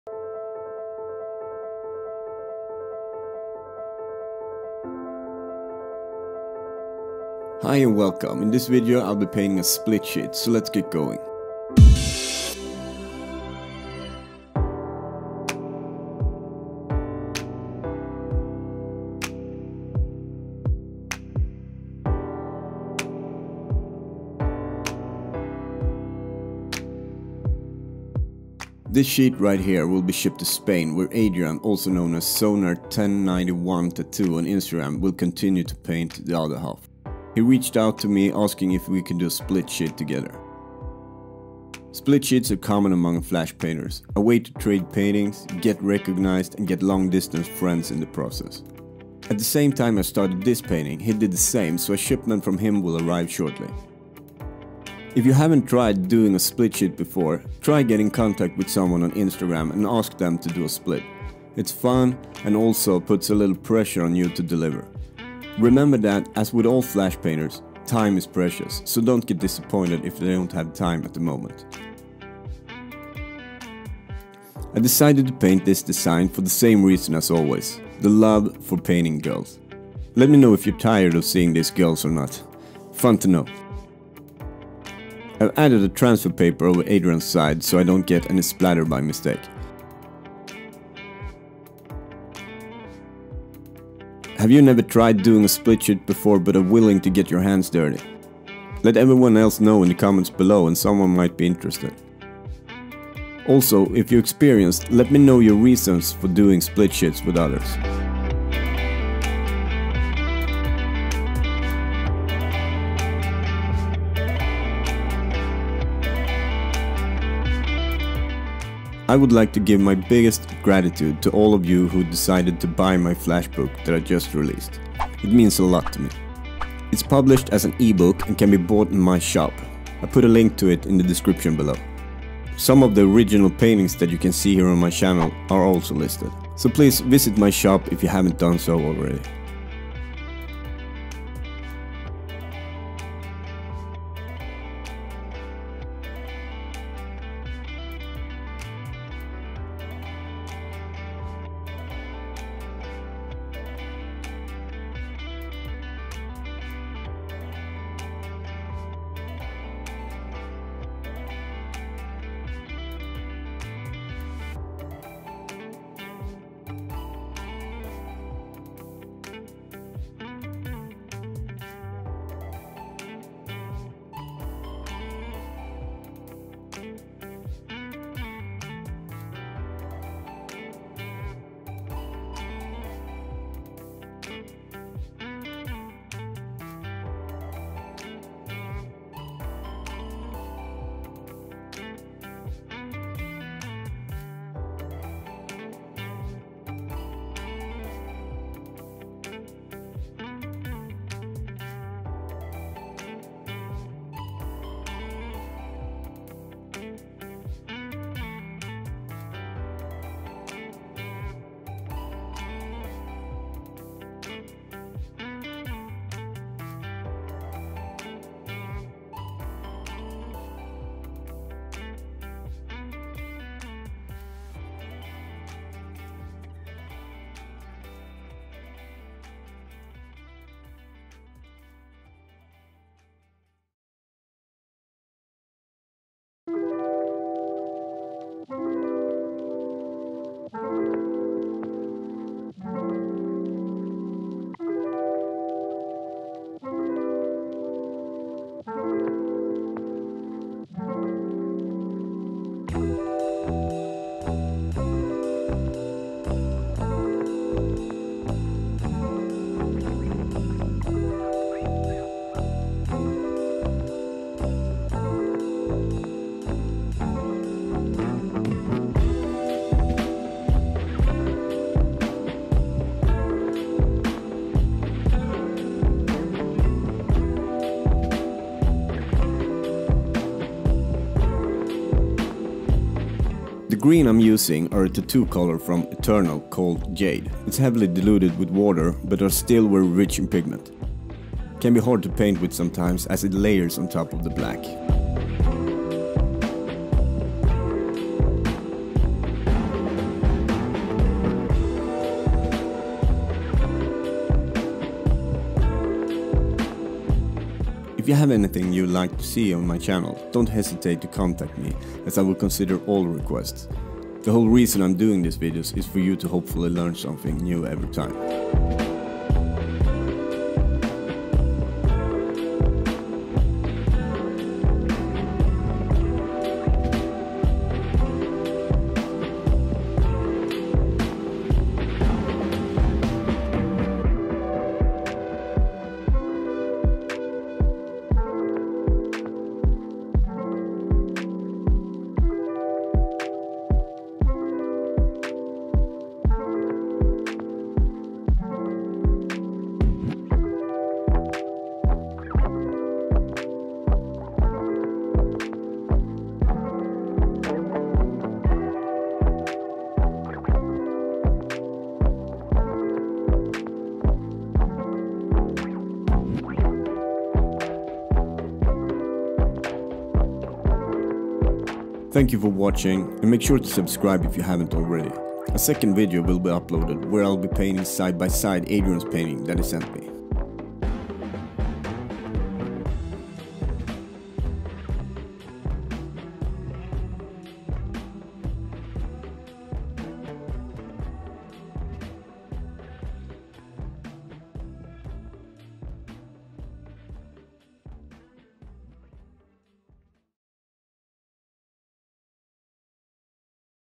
Painting a Traditional Tattoo Flash [Split Sheet]
Talking about split sheets while painting my side of this split that I'm doing together with Sonar1091Tattoo on Instagram.

Go check him out as he will release the painting that will be sent to me.